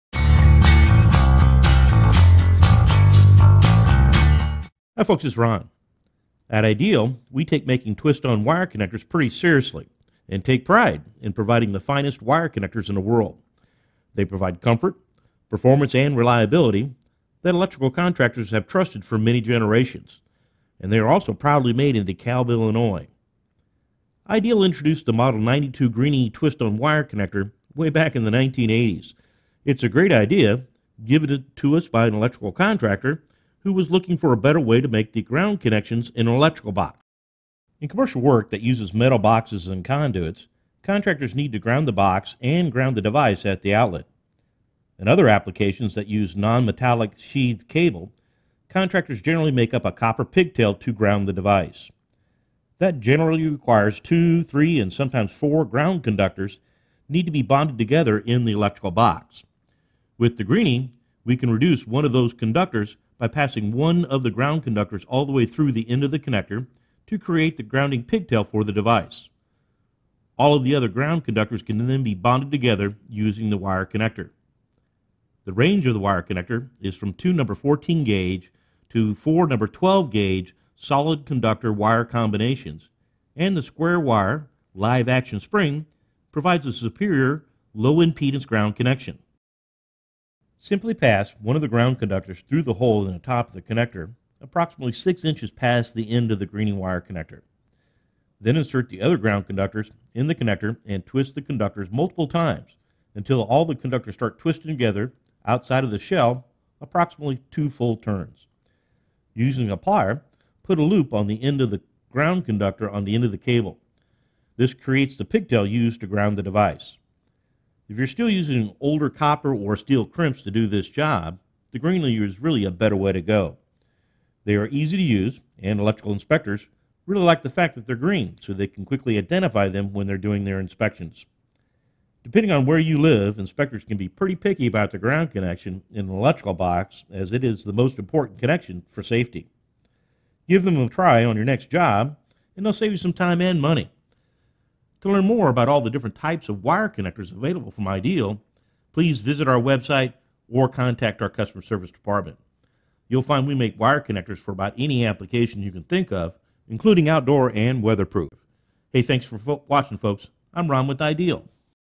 IDEAL 30-092 Greenie Grounding Wire Connector
Join Ron Kipper, IDEAL’s product expert, in learning about the product features and how-tos of the IDEAL Electric products including these Greenie wire connectors. They are UL Certified safety rated. Want to learn more about the family of IDEAL products?